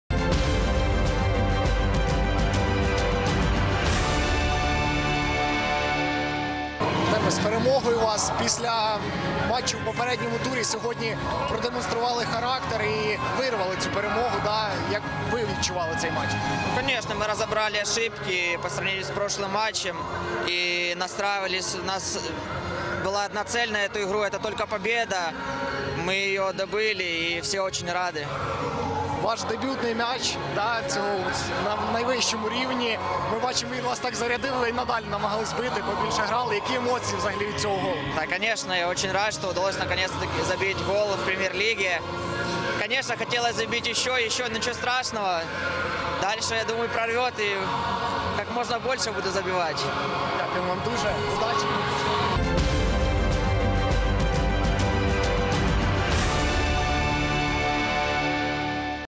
Артем Полярус прокомментировал свой дебютный мяч в Премьер-лиге
Игрок Александрии прокомментировал непростую победу своей команды над одесским Черноморцем в 19 туре ЧУ 2015/2016 и поделился впечатлениями от первого забитого гола в Премьер-лиге.